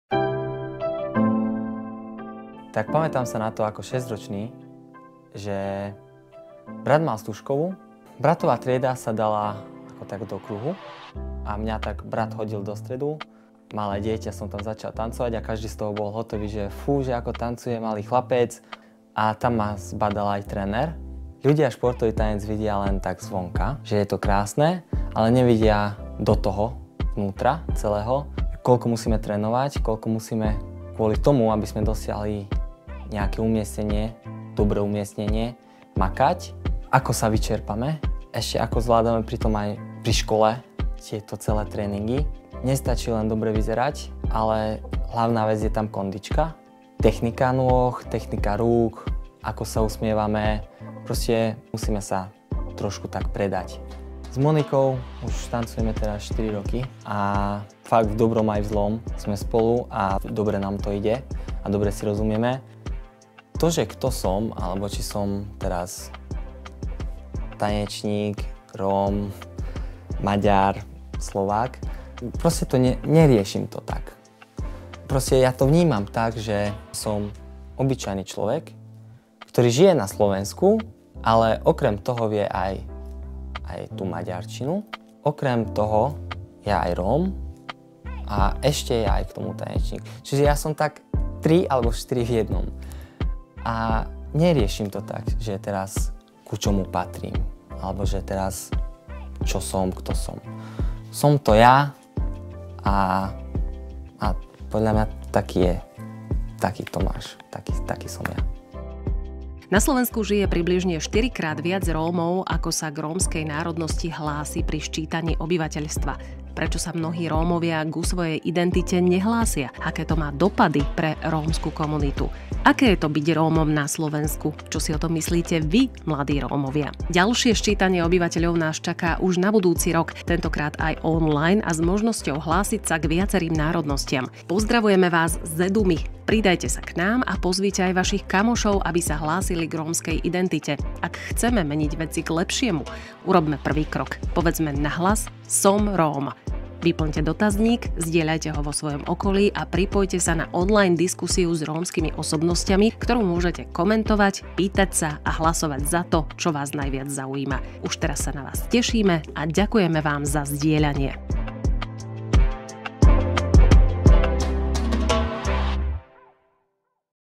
Kto som? Odpovedá Tomáš Kaluja
Som Róm, Slovák alebo Maďar? Otázka, ktorú si možno kladú tí, ktorí sa cítia byť súčasťou viacerých národností, ak majú vyplniť kolónku národnosť pri Sčítaní obyvateľov. Ako to cíti Tomáš, si môžete pozrieť vo videu.

Rómovia ako súčasť Slovenska je projekt podporený Fondom na podporu kultúry národnostných menšín.